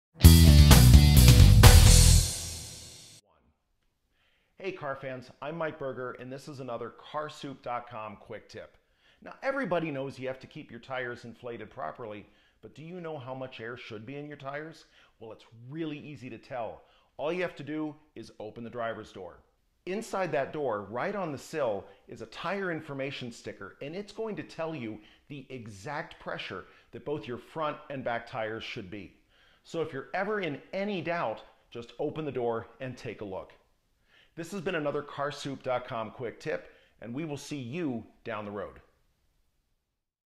CarSoup Quick Tip--Maintain Proper Tire Pressure
Know what the proper tire pressure should be for your vehicle's tires? Just open the driver's door to find out!